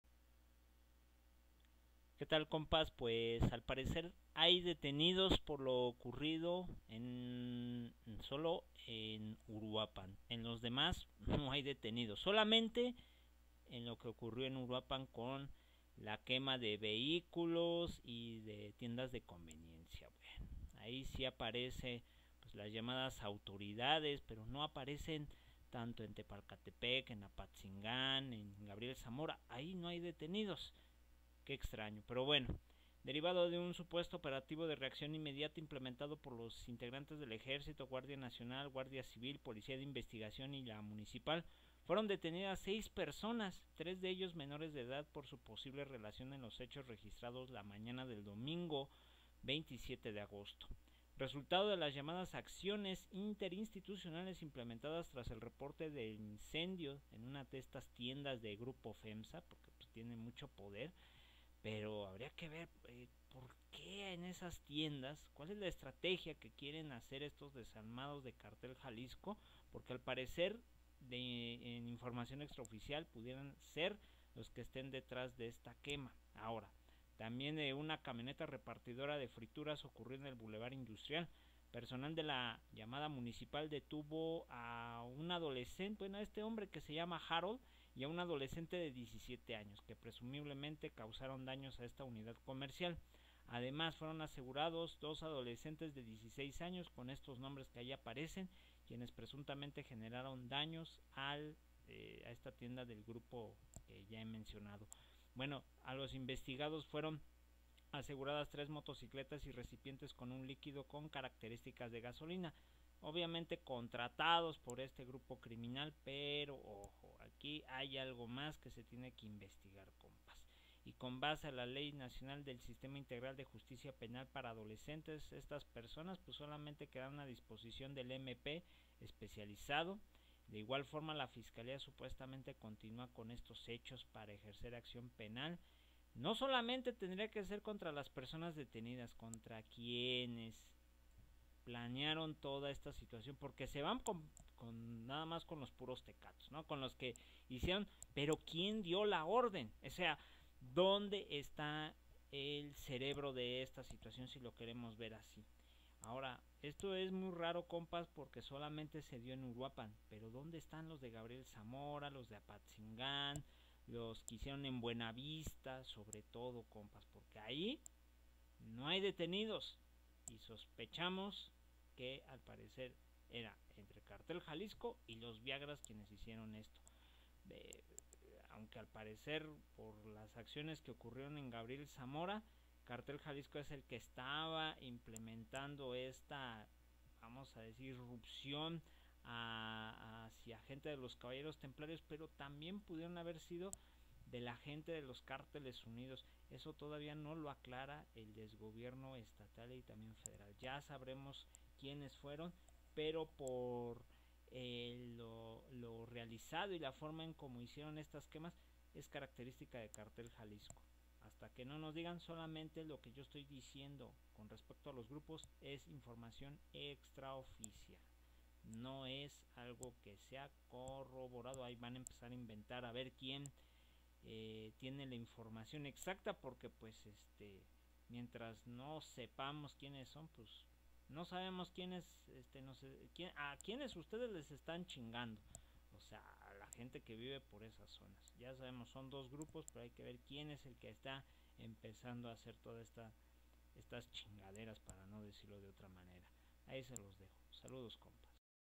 Michoacán Detienen a seis tras hechos ocurridos en Uruapan, la mañana del 27 Agosto 2023
Foto: FGE Michoacán